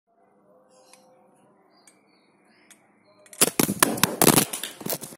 Hp jatuh saat buat konten saat rehat ekspedisi
saat mau buat konten hp jatuh suasana jadi ambyaaaar